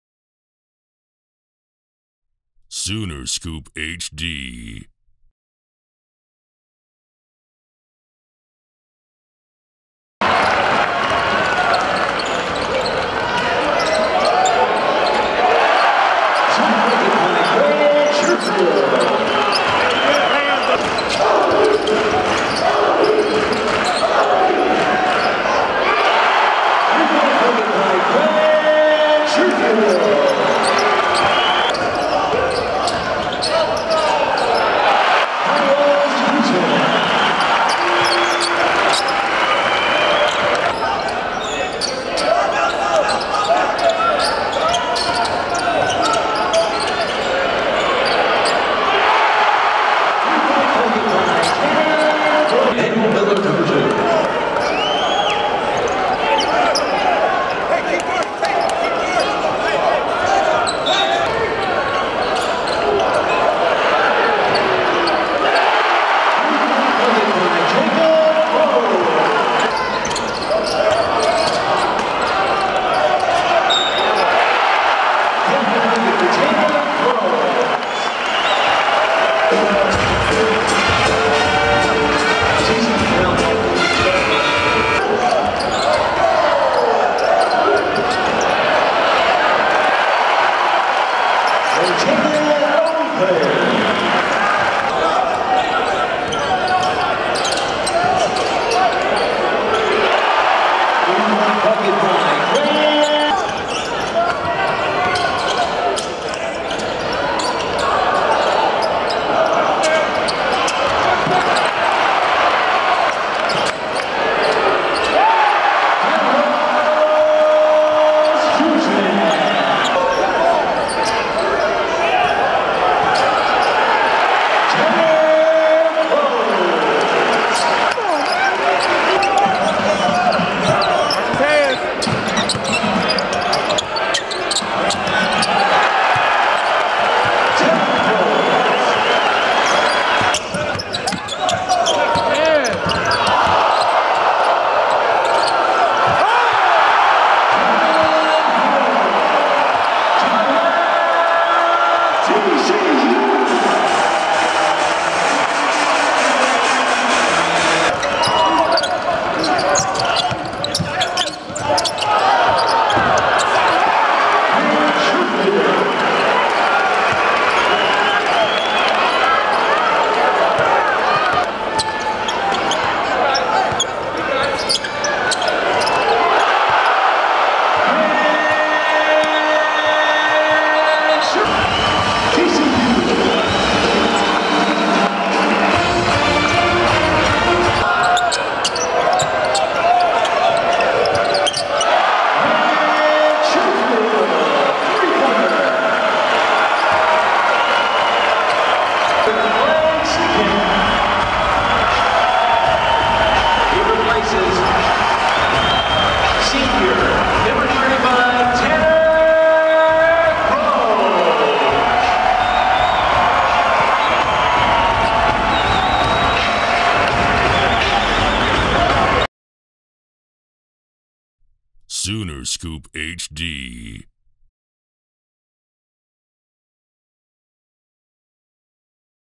HOOPS: Sights & Sounds - TCU
SoonerScoop.com's most popular video feature SCOOPHD Sights & Sounds highlights the Sooners regular season finale victory versus TCU.